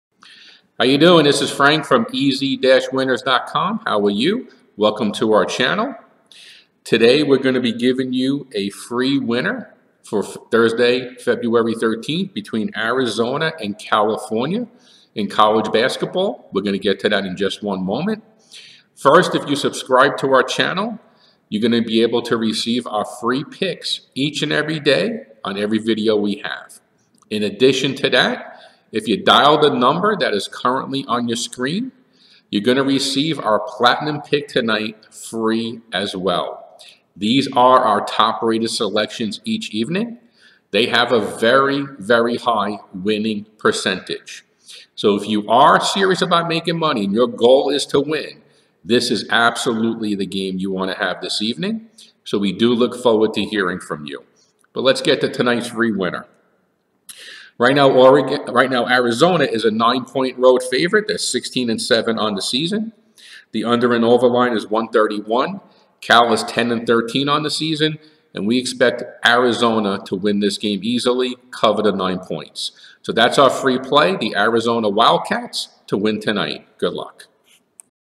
College Basketball Picks Arizona vs California Free Prediction 2/13/20
Arizona wildcats vs California Bears free college basketball prediction 2/13/20. If your current NCAAB betting picks have you down checkout ez-winners for a free under/over and points spread pick. Zona 16-7 su 12-11 ats are 9 point road underdogs at Cal 10-13 straight up 10-12 against spread with a totals line of 131